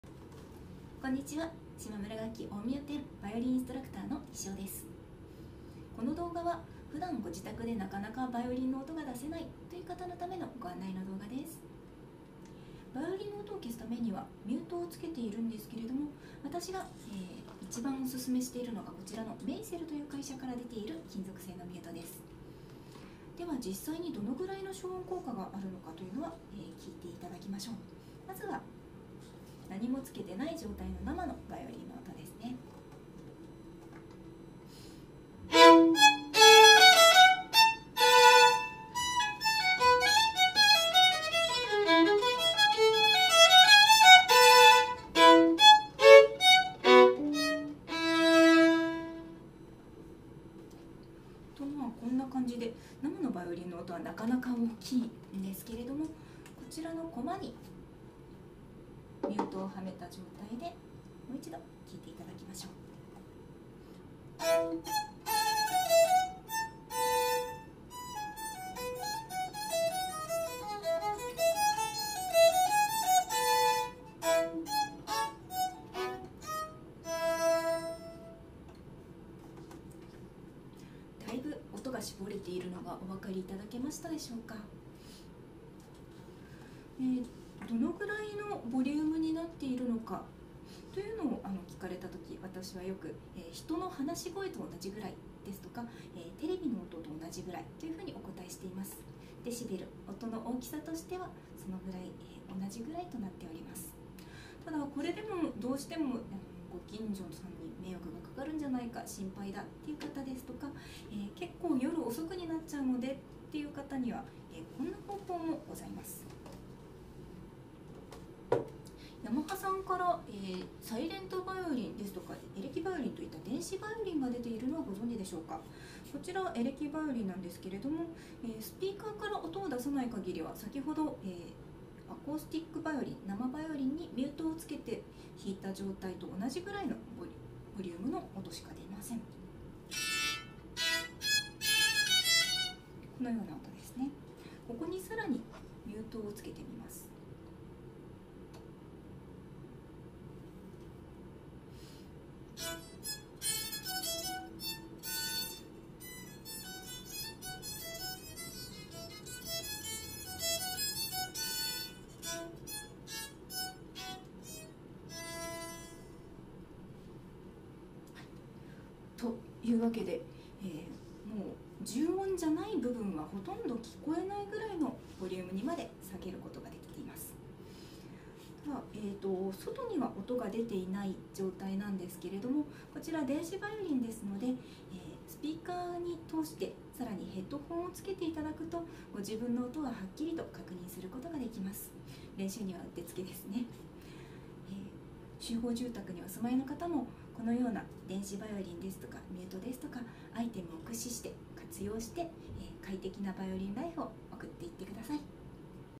【島村楽器大宮店バイオリン】ミュートとエレキバイオリンを試してみた
バイオリン用金属ミュートの消音効果のご紹介です。

ご案内：島村楽器大宮店バイオリンインストラクター 西尾
使用ミュート：ヘビープラクティス VM-1
使用EV：YAMAHA YEV-104

通っている会員さんの中にも、生音を出して練習する環境がないとお悩みの方がいらっしゃいます。
他にもバイオリンの演奏について気になることがあればご相談ください！

●バイオリンの体験レッスンは随時受け付けております。
入会金不要の１ヶ月おためしレッスンも開催中です♪